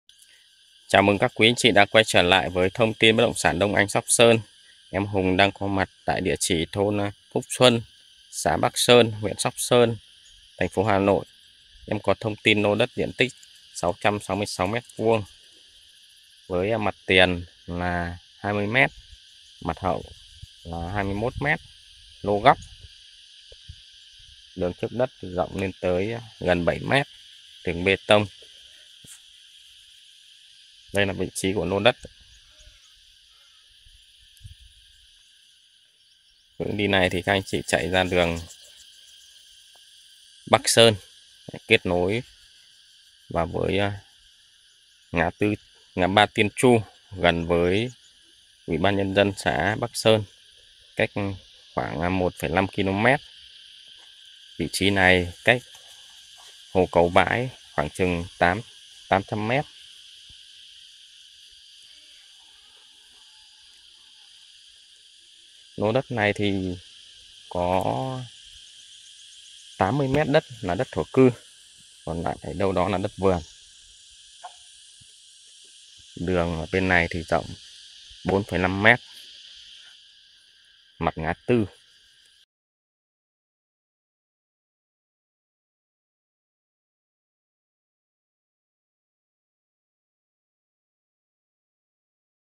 Lô góc trục chính Phúc Xuân, Bắc Sơn, Sóc Sơn, HN. Dt 1186m2, Mt 43m
Đường trục chính 2 ô tô tránh, cách hồ Cầu Bãi chỉ hơn 200 mét, ngay cạnh khu nghỉ dưỡng chuồn chuồn cực kỳ đẳng cấp

Zalo Mr Vương Hùng
❤️ Đo đạc đất đai
❤️ Hỗ trợ vốn